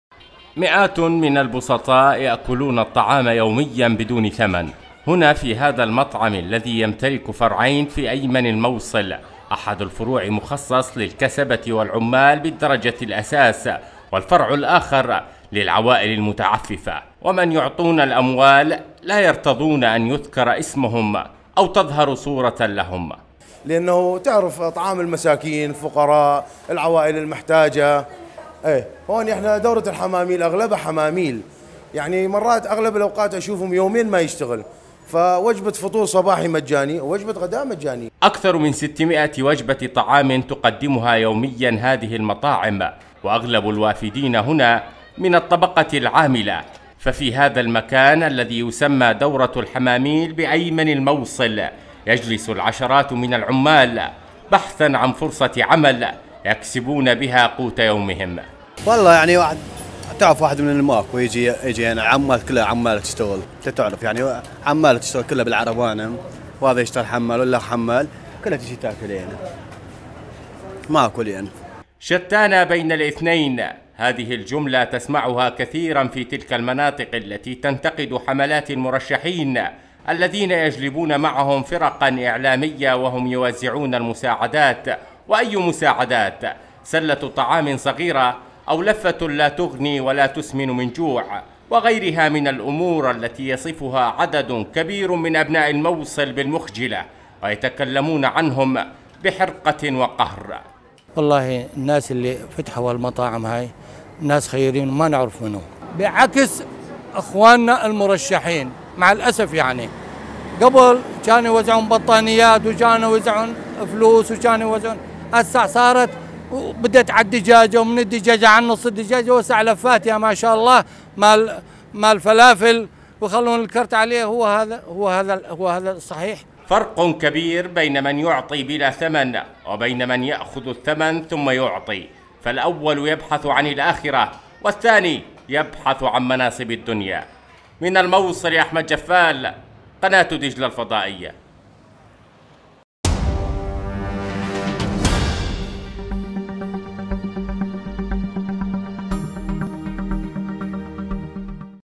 انتقادات للمرشحين الذين يصورون توزيع المساعدات في الموصل
10891📺 افقي
10892📺 افقي
10893📺 افقي